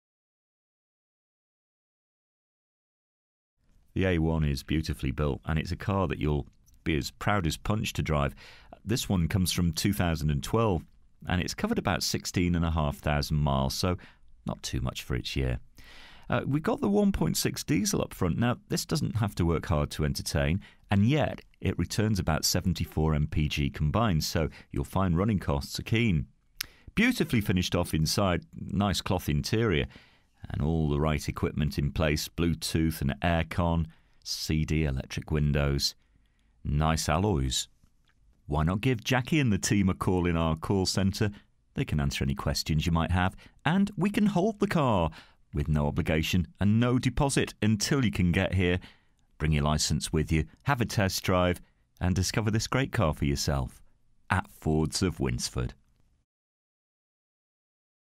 AUDI A1 DIESEL HATCHBACK (2012) 1.6 TDI SPORT 3DR - MT61XSD
DETAILS:
First registered: 06/01/2012
Current mileage: 16640
Registration: MT61XSD
Colour: BLUE

FEATURES:
front electric windows, remote central locking, cloth interior, all round airbags, air conditioning, pas, abs, alloy wheels, metallic paint, cd, traction control, parcel shelf present, hand book present, bluetoot

This used car is for sale at Fords of Winsford Car Supermarket in Cheshire, UK